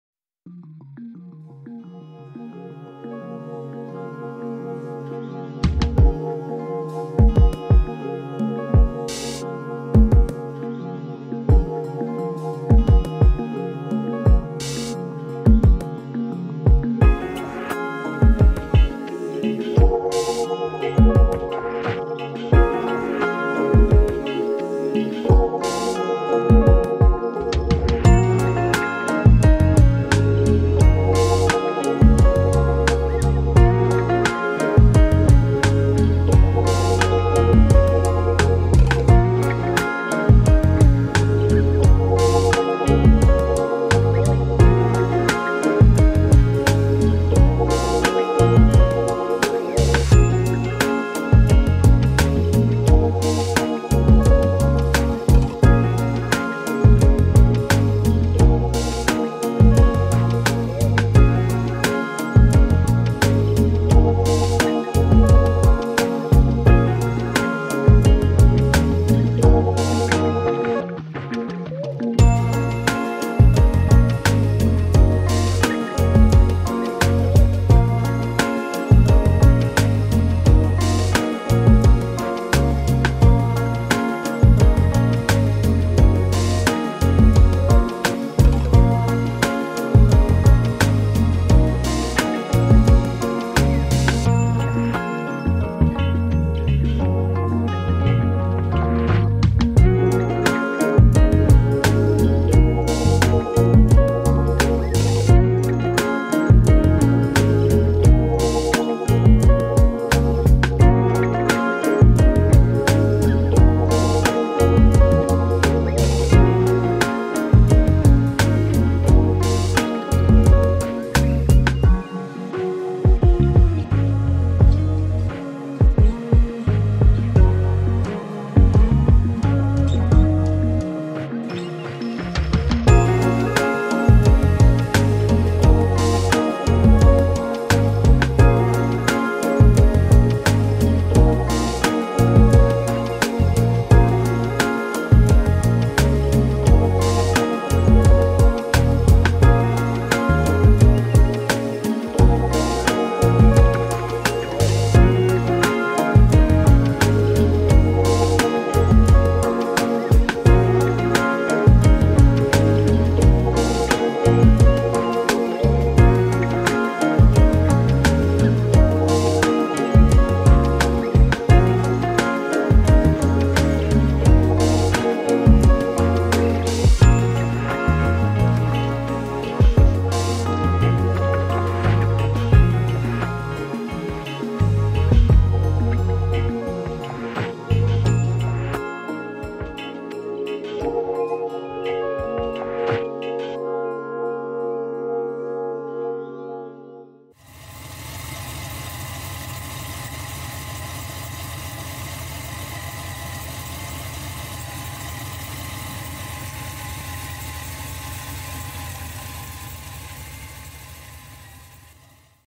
1971 Chevrolet Nova at Hot Rodders for Hooters Car Show by Racin Repair Inc
1971 Chevrolet Nova at the Hot Rodders for Hooters Car Show at Rockwoods by Racin Repair Inc in Otsego Minnesota on Sunday May 5th 2024 .